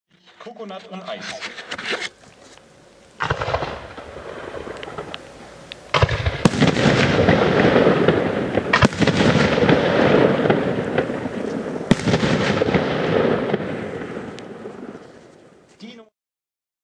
(66) Blackboxx - Coconut & Ice (100 mm)
3 Stück. Gezeigt wurde dieser Artikel auf der Produktpräsentation von Blackboxx am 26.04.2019 in Wolkenstein.